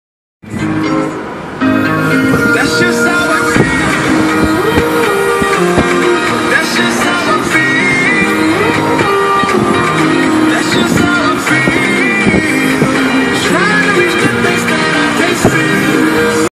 Tiger, the poodle feels
Tiger the Poodle enjoys the wind in his hair.  So cute!